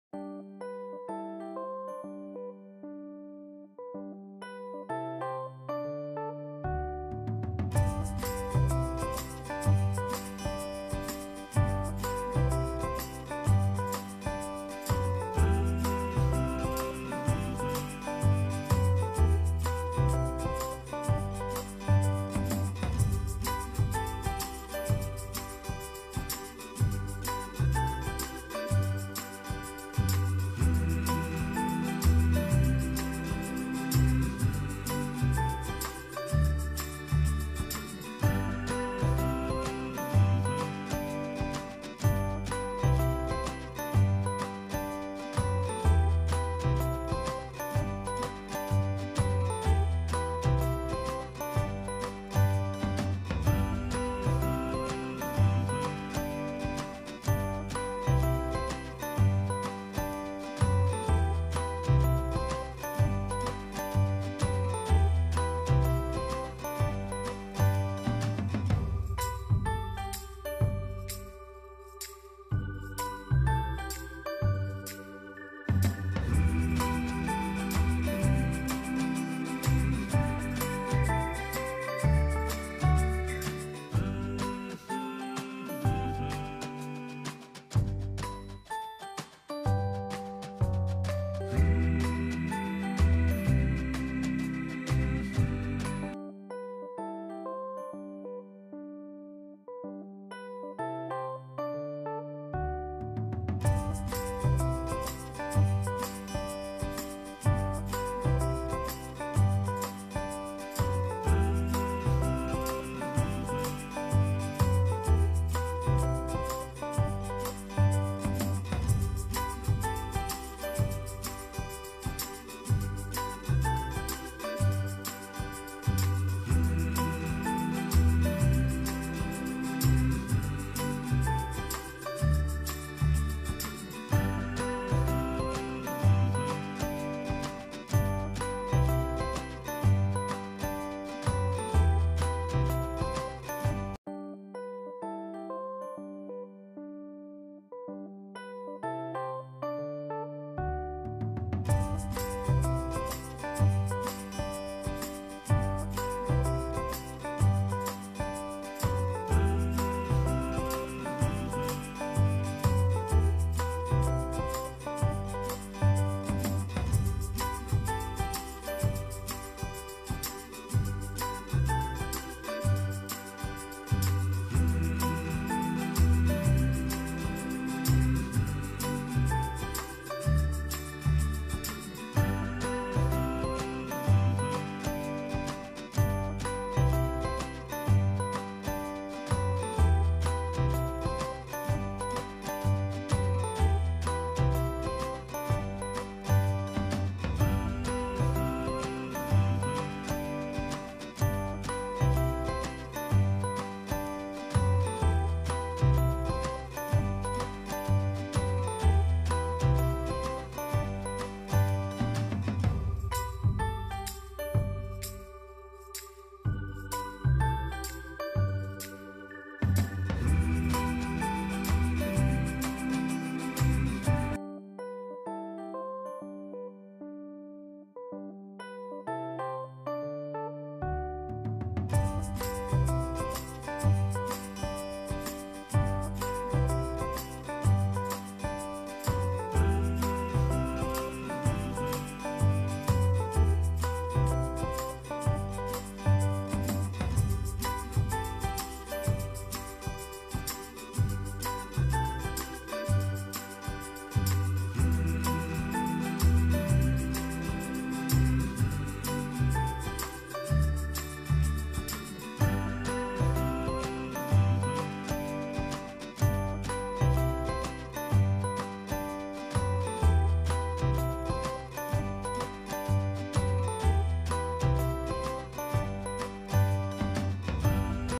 carrot pudding|carrot milk pudding|No bake eggless carrot milk pudding|pudding recipes
Hai friends

         In this video we going to see about making of carrot pudding